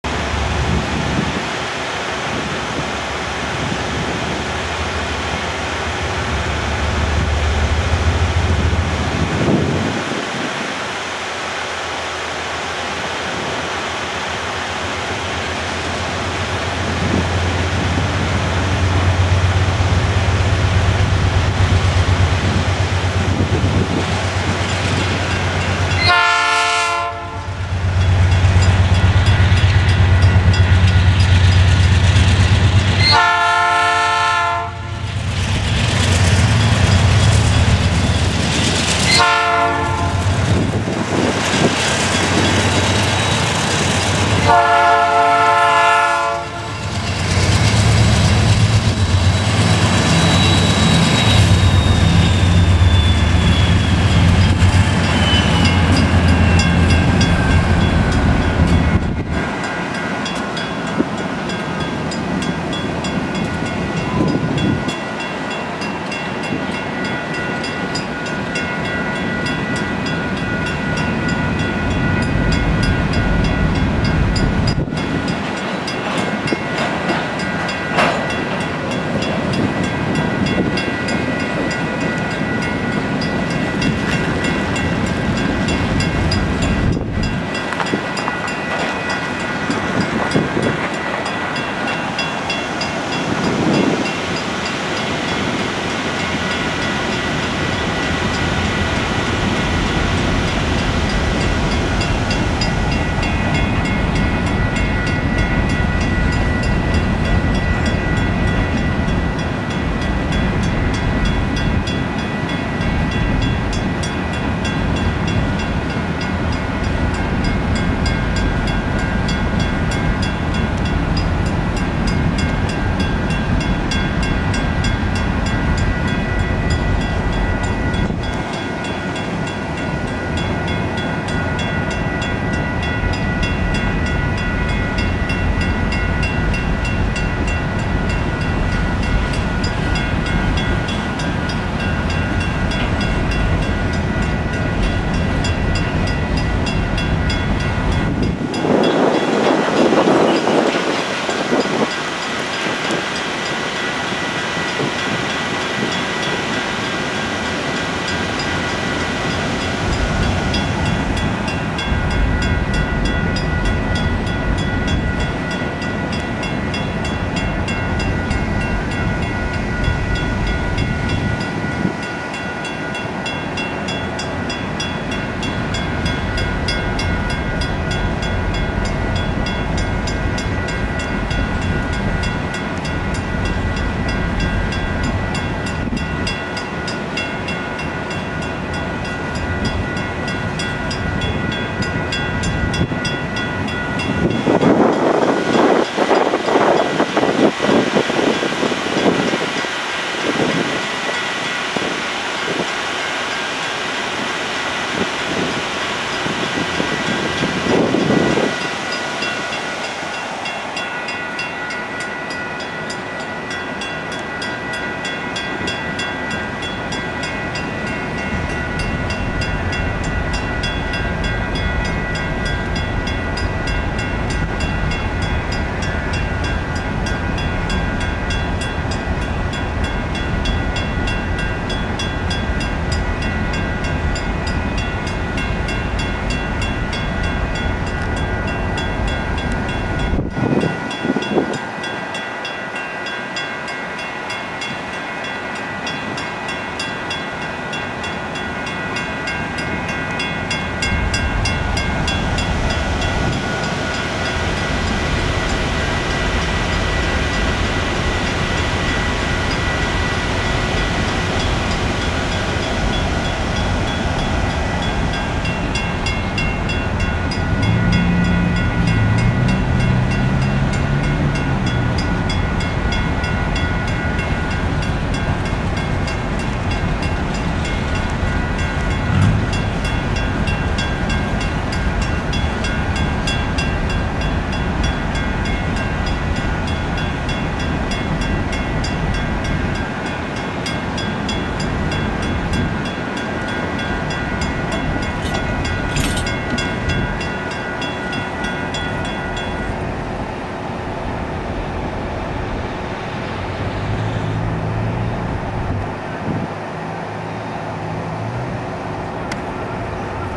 UP 9683 leads westbound rock train out of Rosenberg, TX
A westbound UP rock train is starting to leave the Rosenberg, TX area and is more likely heading toward Sealy, TX via the BNSF Galveston Sub in order to get to the UP Smithville Sub.

UP C44-9W #9683
UP C44-9W #9649
UP C44-9W #9603